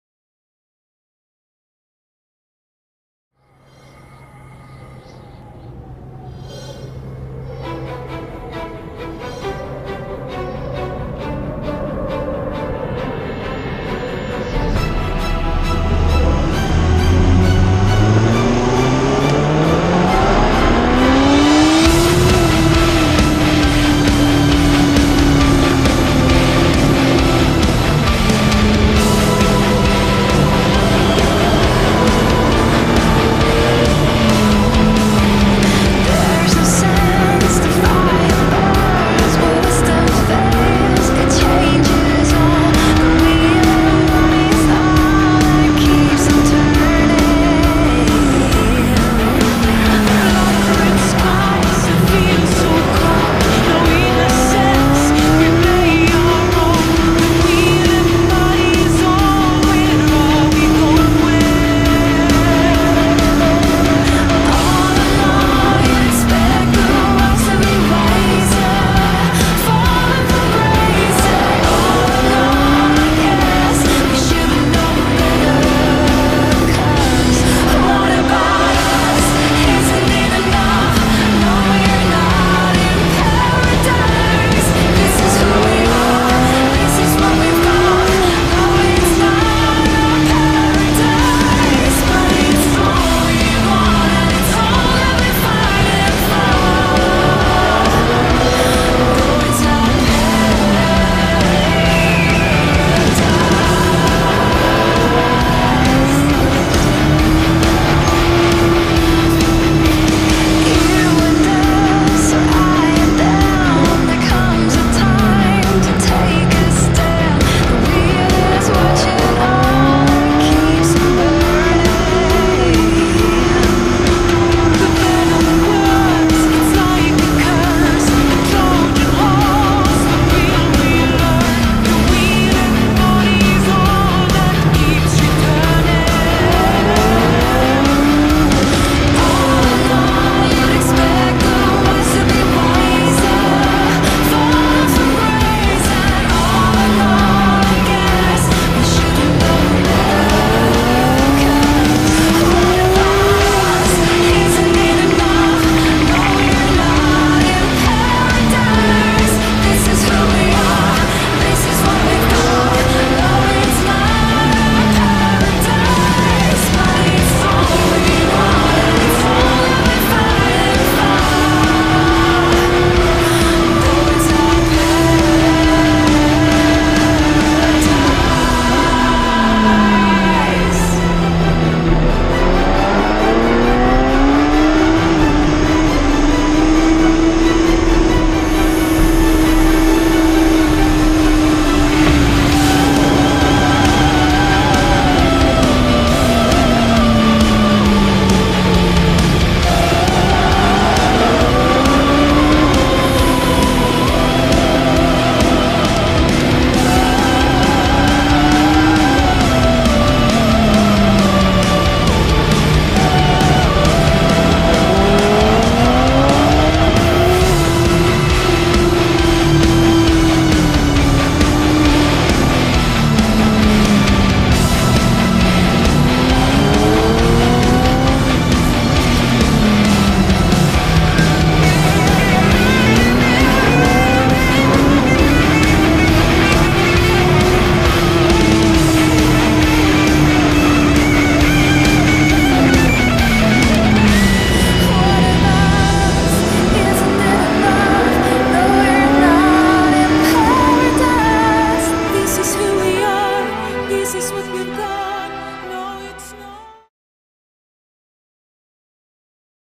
vaison piste 23 05 2021, goret team motorsport, Arnaud David #423, Kawasaki zx10r
un petit tour à Vaison piste en Kawasaki zx10r !